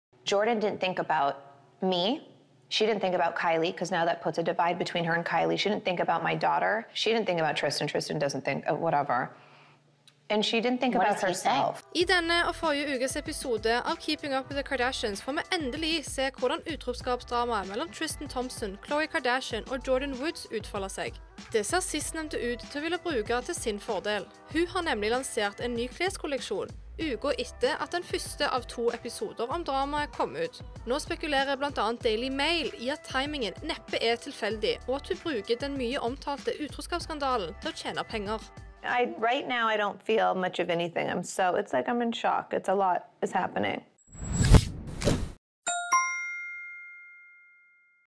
Anklages for å tjene penger på utroskapsskandalen
PENGER: Flere spekulerer i om timingen av Jordyn Woods nyeste kleskolleksjon er tilfeldig. Video: TV2 Sumo. Reporter: Ingebjørg Iversen / Rød Løper